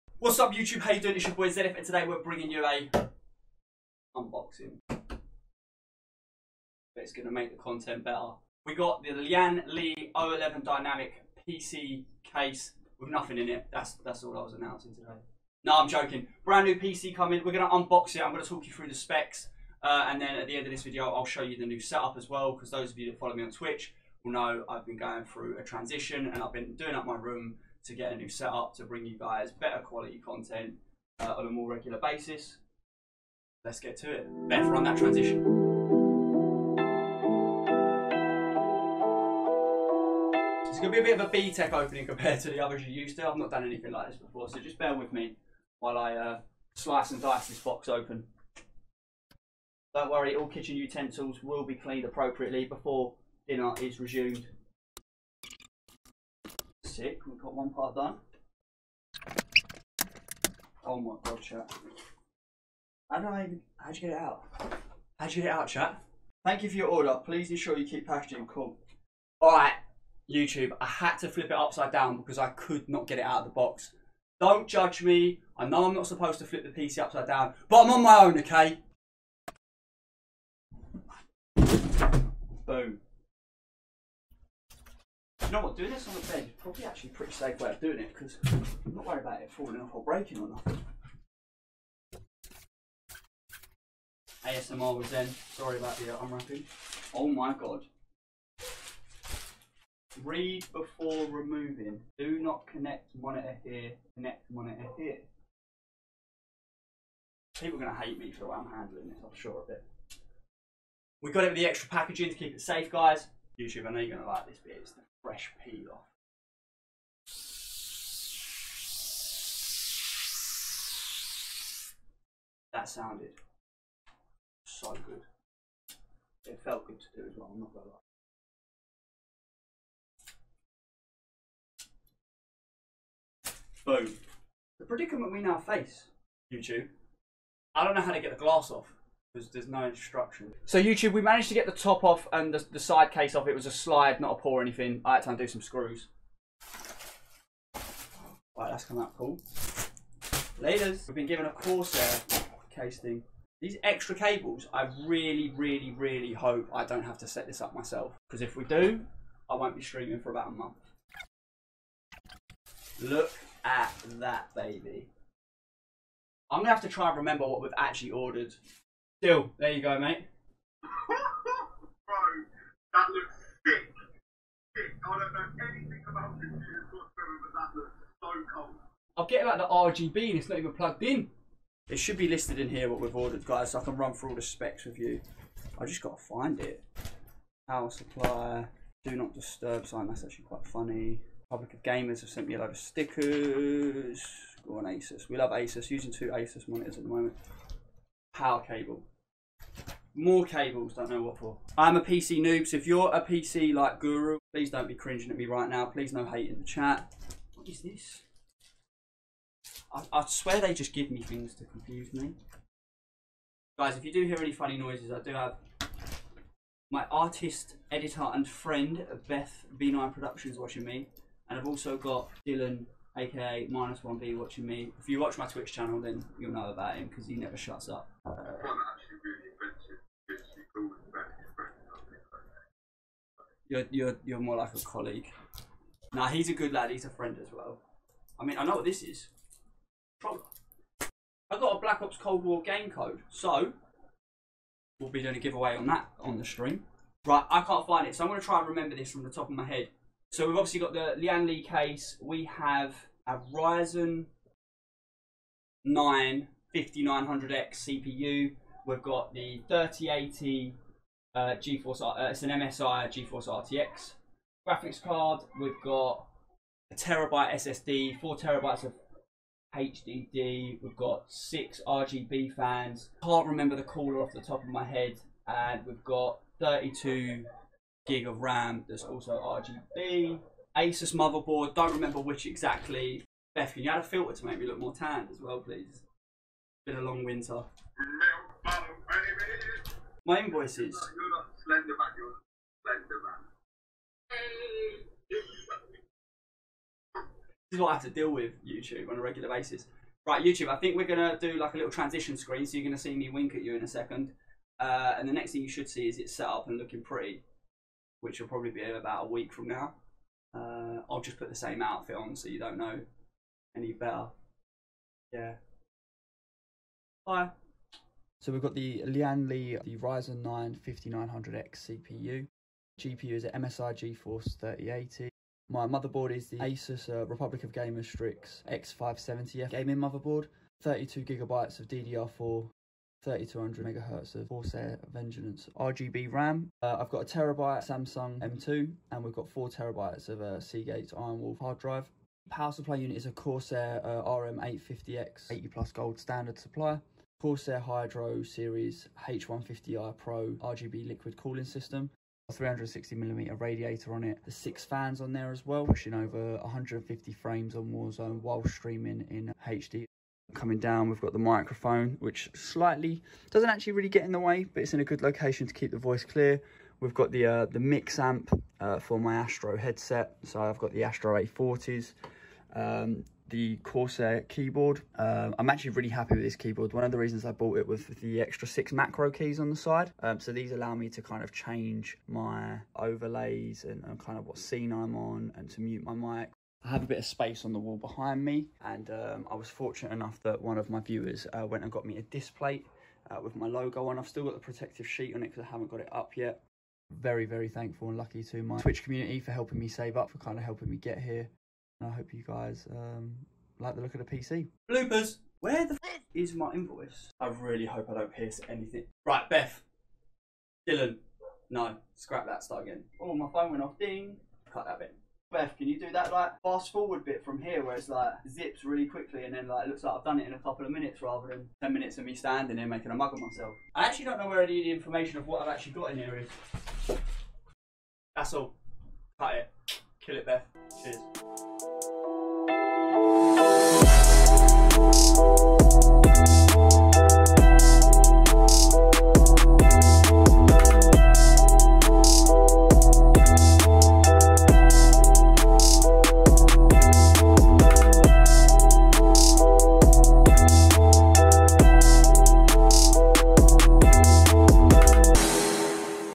Brand NEW PC Unboxing & Gaming / Streaming Set Up Tour
PC Specs:
Case: Lian Li O11 (Black)
CPU: Ryzen 9 5900X
GPU: MSI GeForce 3080 10GB
Mobo: ASUS ROG Strix X570-F Gaming
RAM: 32GB DDR4 3200MHZ Corsair Vengeanance Pro RGB
M2: 1TB Samsung 970 Evo Plus
HDD: 4TB Segate IronWolf Pro Sata-III
PSU: Corsair RM850x 80+ Gold
Cooler: Corsair Hydro Series H150i Pro XT RGB Liquid Cooling System w/ 360mm Radiator
Fans: ​6x 120mm Cyberpower Addressable Dual Light Loop RGB

Set-up:
Main Monitor: ASUS TUF Gaming VG279QM
Left Monitor: ASUS VG245H
Right Monitor: AOC 24G2U5
Arm Mounts: Bontec Single Monitor Arm Desk Mount & Bontec Dual Monitor Stand
Webcam: Sandstrom SWCAMHD19 Full HD Webcam
Headset: Astro A40 w/ Mix Amp Pro TR Gen 4
Microphone: UHURU Professional PC Streaming Mic
Mouse: Corsair M55 Pro RGB Gaming Mouse
Keyboard: Corsair K55 RGB Gaming Keyboard (6 Macro Keys)
Controller: Xbox Elite Wireless Controller Series 2
Ringlight: 2x Docooler 6inch Ring Light with Stand
Clock: PICTEK Digital Alarm Clock